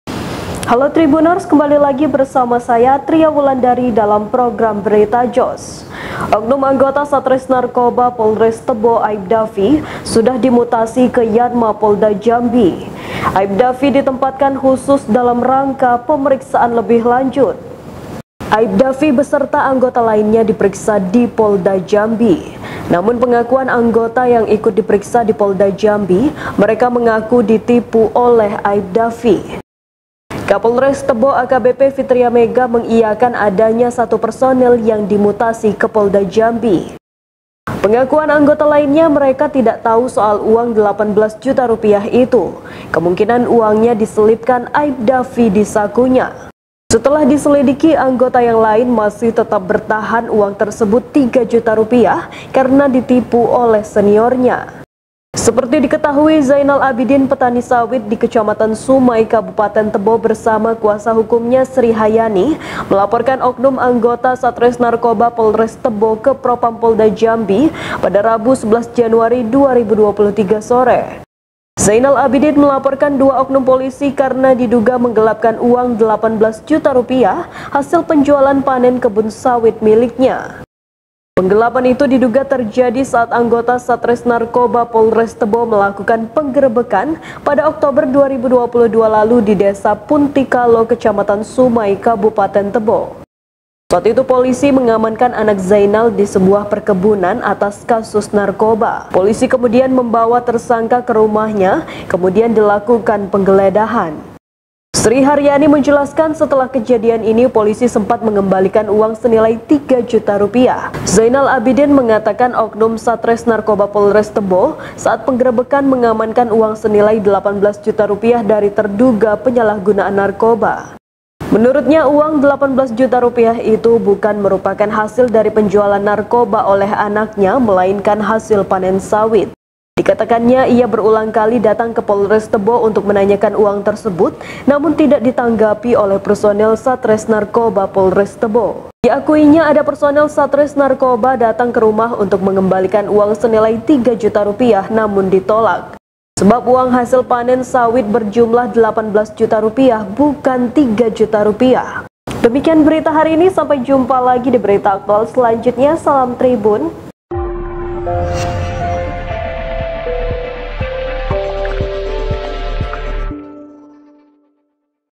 Aipda V Oknum Anggota Polres Tebo Akui Selipkan Uang Rp 13 Juta Saat Penggerebekan Kasus Sabu
Oknum anggota Satresnarkoba Polres Tebo Aipda V sudah dimutasi ke Yanma Polda Jambi.

Aipda V ditempatkan khusus (Patsus) dalam rangka pemeriksaan lebih lanjut. (ria)